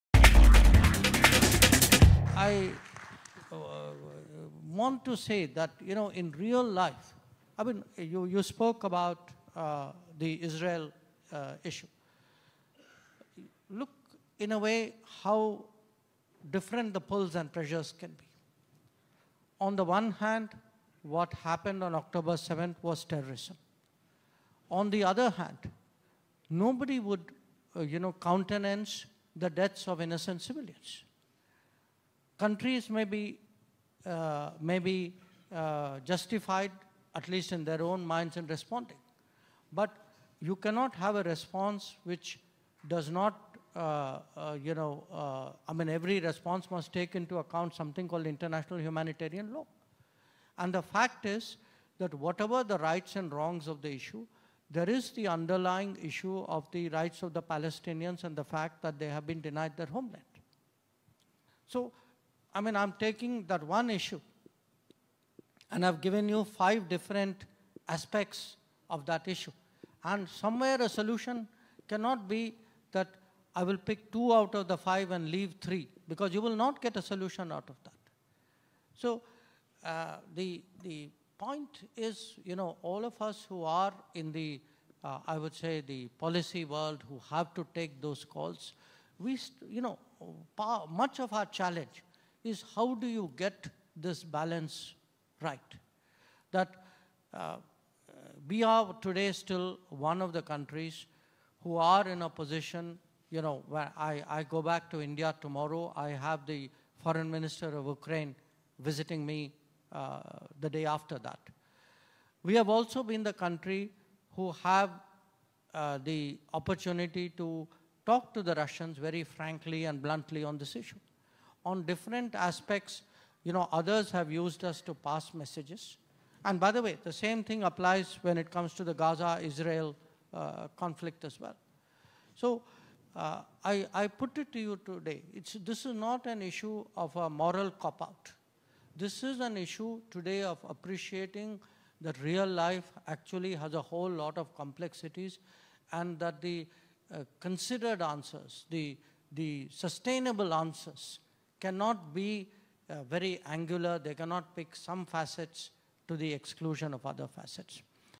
Israel Vs Hamas | EAM Jaishankar Makes A Strong Statement On Israel-Palestine Conflict | N18V
External affairs minister S Jaishankar has expressed concern over the death of civilians in the ongoing Israel-Hamas war stating that Palestinians have been denied their homeland. He also urged Israel to cater to international humanitarian law in its ongoing retaliation after the deadly October 7 attack by Hamas. However, S Jaishankar acknowledged that the October 7 attack was “terrorism”.